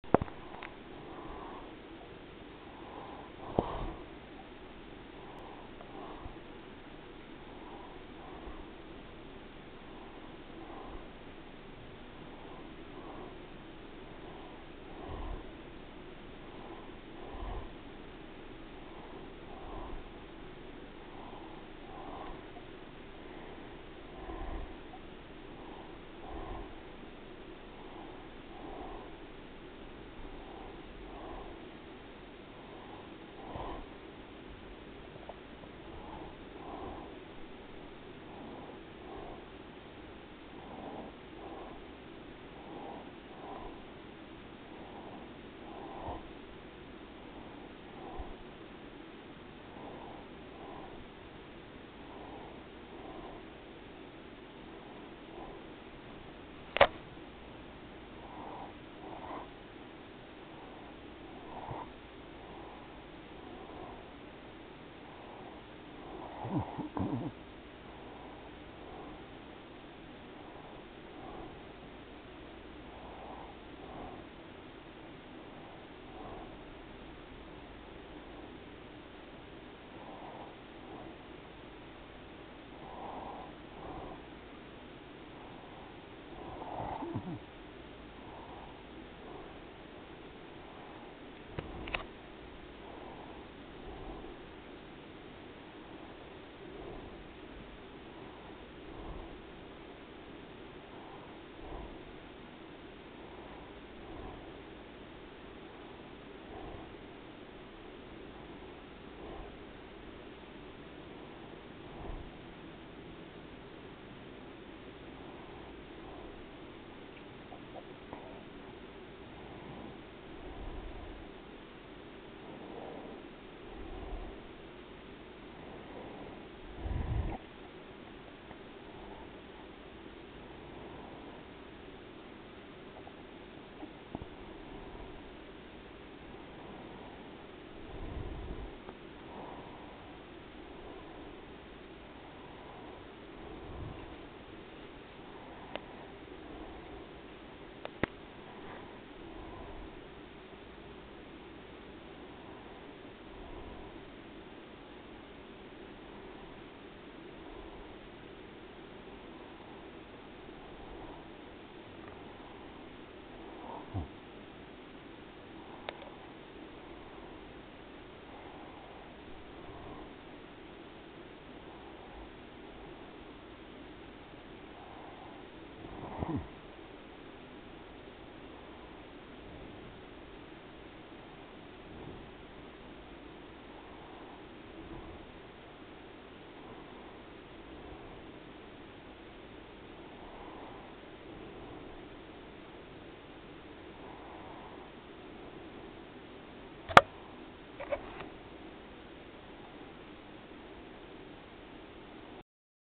Daisy My Cat......
Resting after a long hard day of eating sleeping and purring...